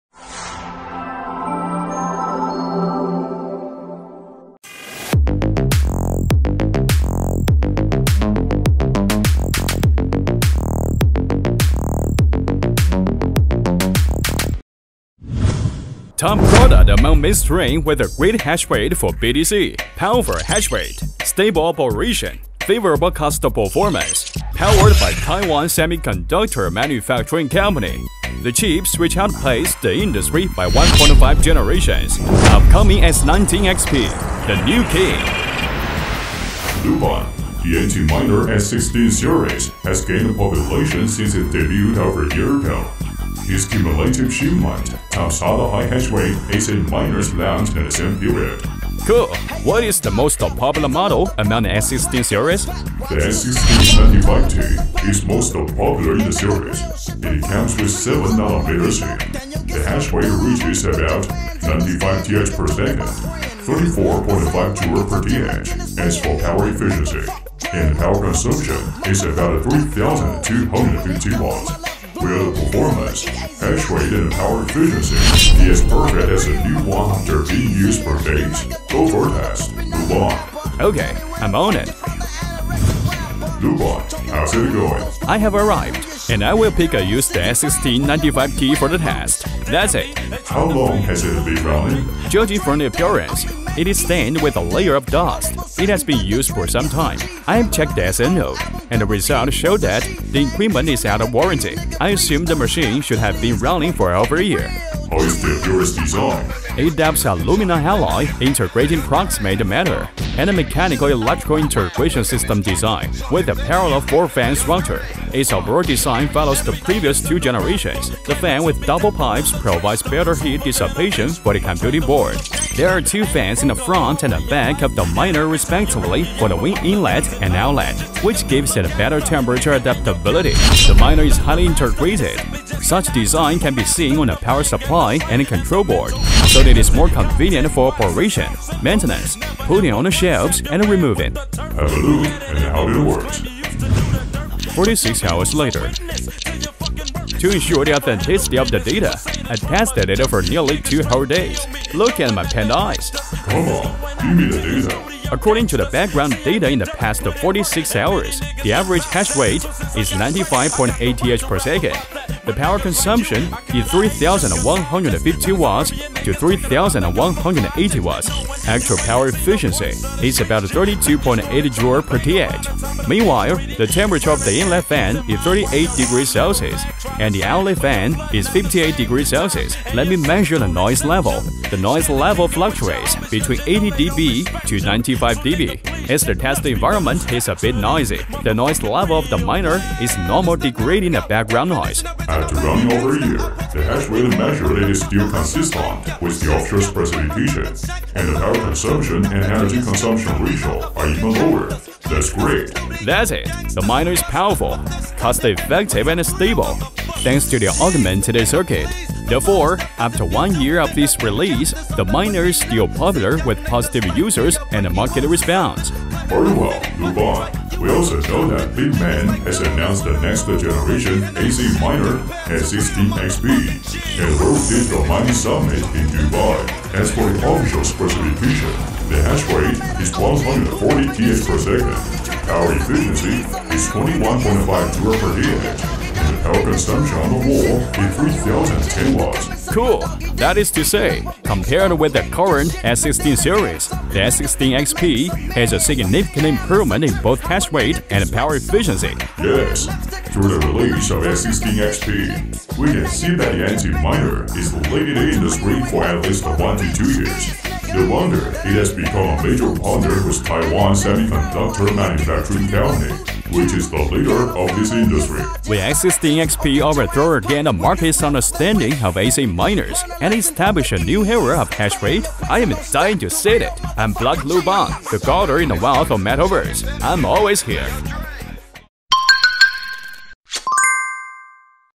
Review of second-hand Antminer S19 95T
After one year of its release, S19-95T is still popular with positive users and market response.The hash rate measured is still consistent with the official specification, and the power consumption and energy consumption ratio are even lower.Bitmain has announced the next generation ASIC miner S19XP at the World Digital Mining Summit in Dubai.Through the release of S19XP, we can see that the Antminer is leading the industry for at least 1-2 years!